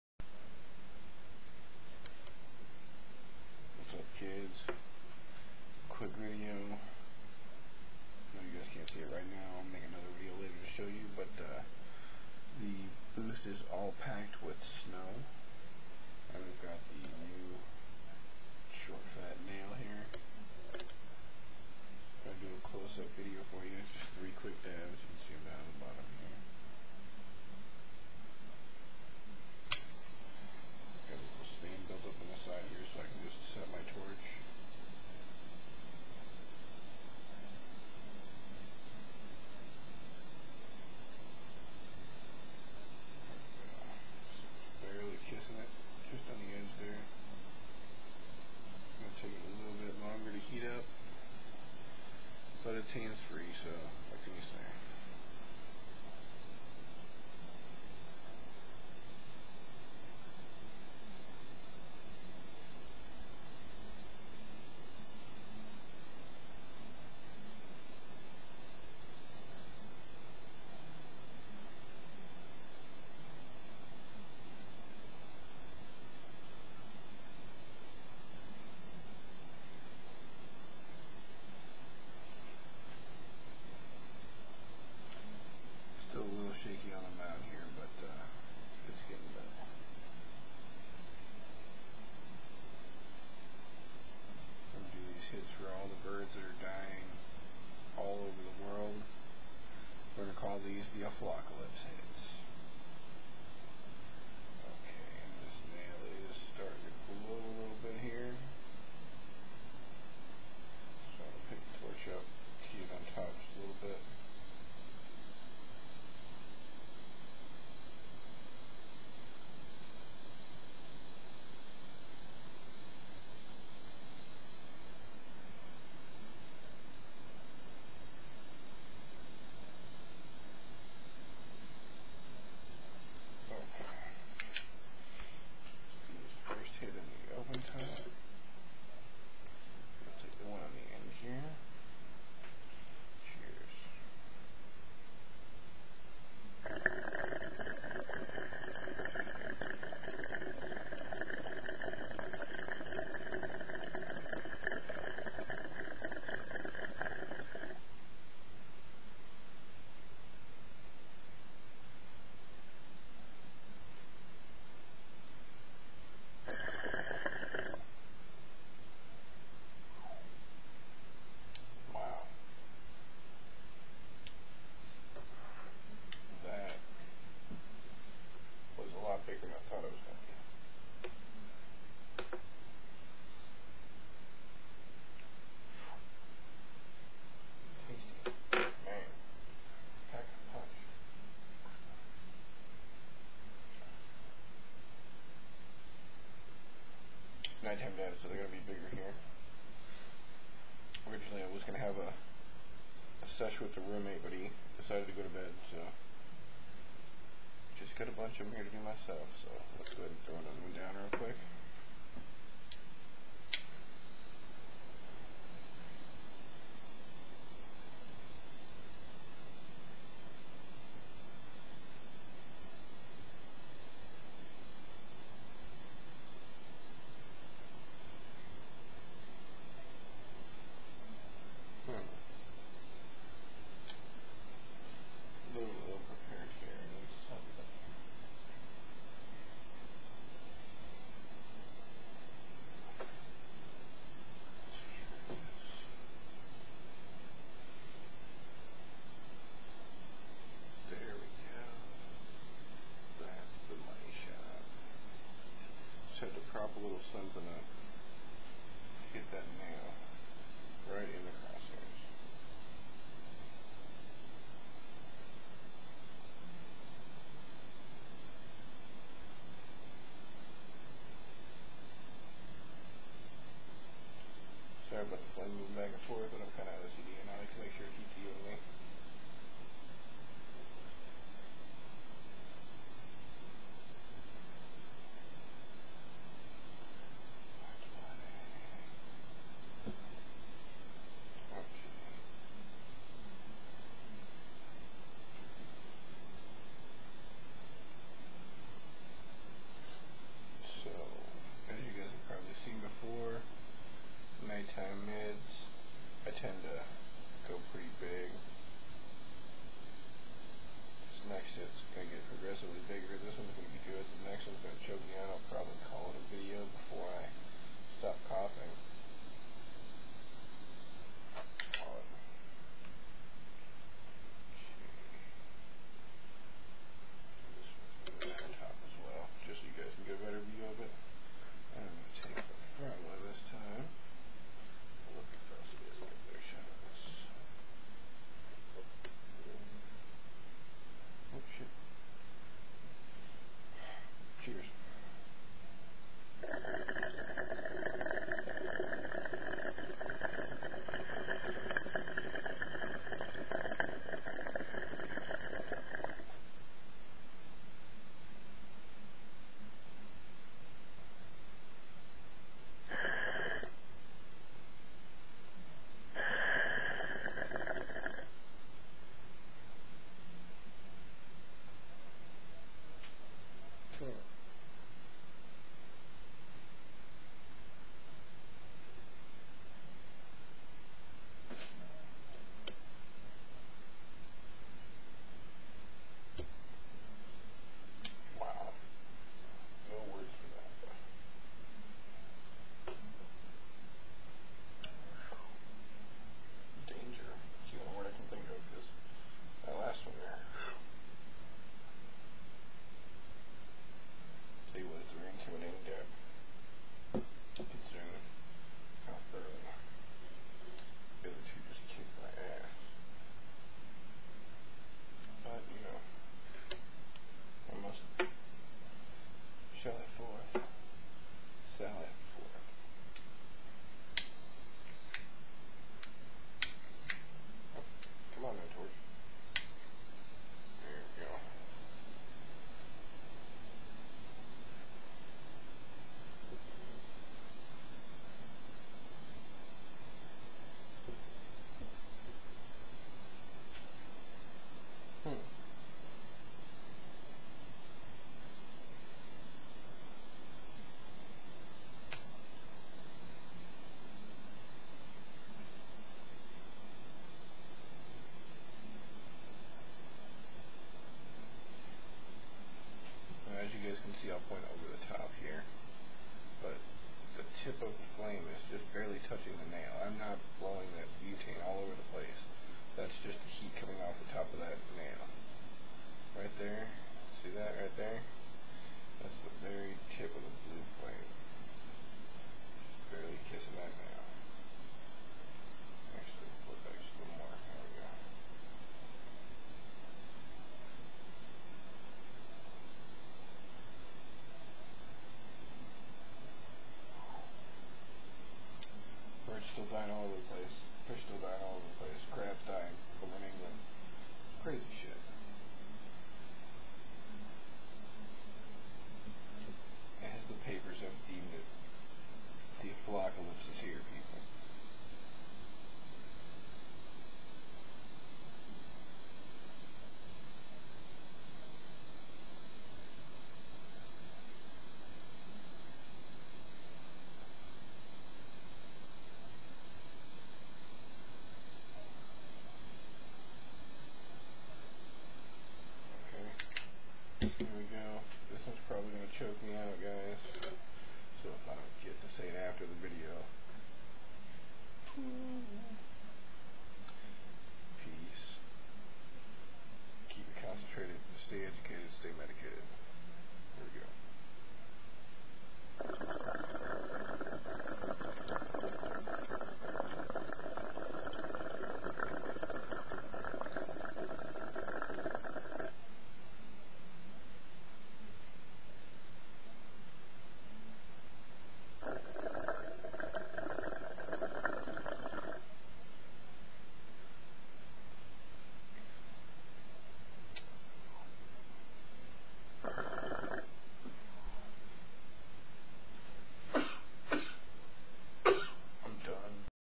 Aflockalypse Dabs For The Animals Dropping Dead Around The World!
A couple of quick dabs for all the animals dropping dead all over the place. Hopefully we find out what is causing this and fix it before it becomes an un-fixable situation. Gotta look out for Mother Nature!